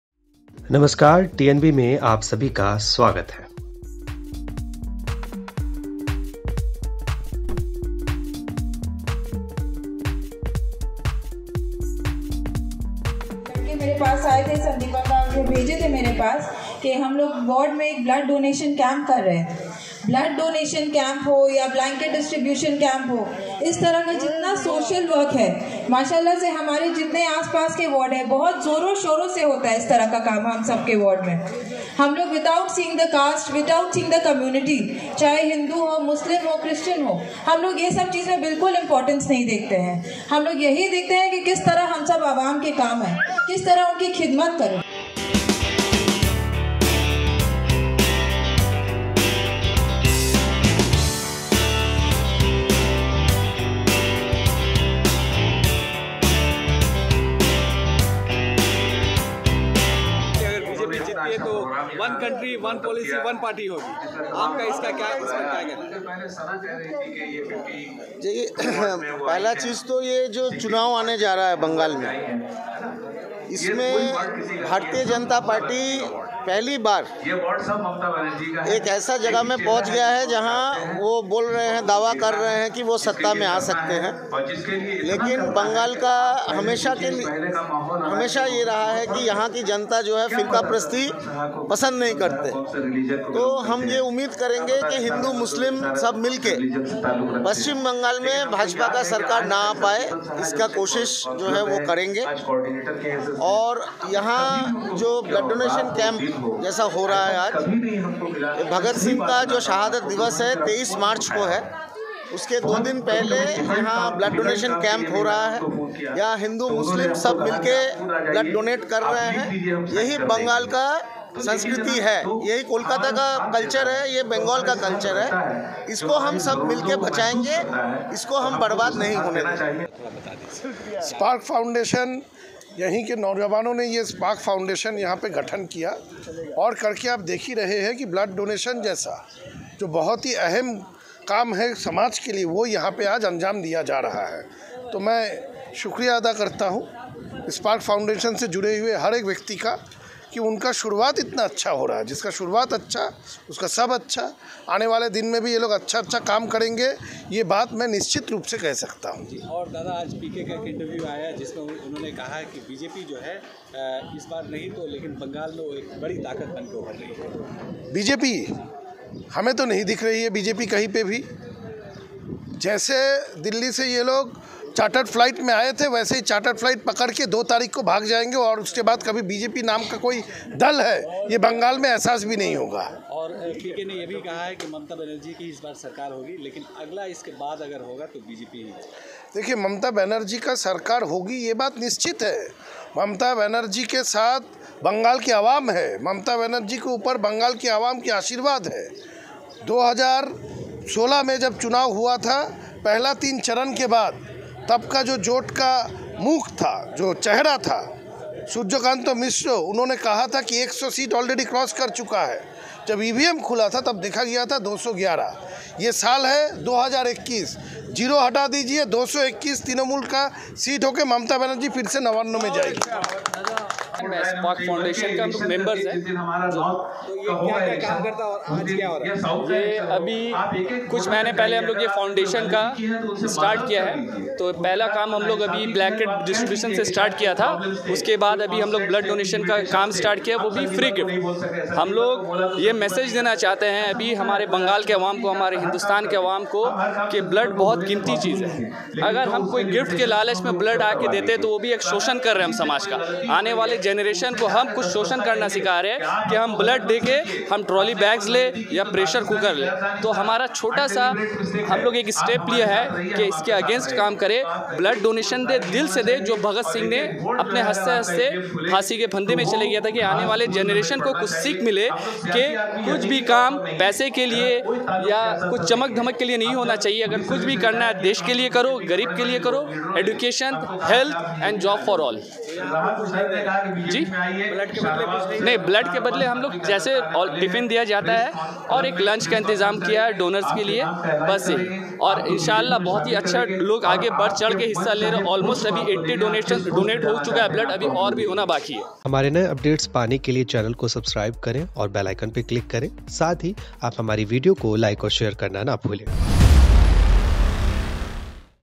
Bengal Election 2021:शहीद Bhagat Singh की याद में Kolkata में हुआ Blood Donation Camp का आयोजन
Shaheed Bhagat Singh की याद में Kolkata के Rafi Ahmed Qidwai Road के Collin Lane पर एक Blood Donation Camp का आयोजन हुआ| Blood Donation Camp में लोगो ने बढ़ चढ़ कर हिस्सा लिया |इस Blood Donation camp का आयोजन Spark Foundation द्वारा कराया गया| Blood Donation Camp में चुनावी रंग देखने को भी मिले| इस चुनावी माहोल में वोटरों को लुभाने की हर कोशिश को अंजाम दिया जा रहा है|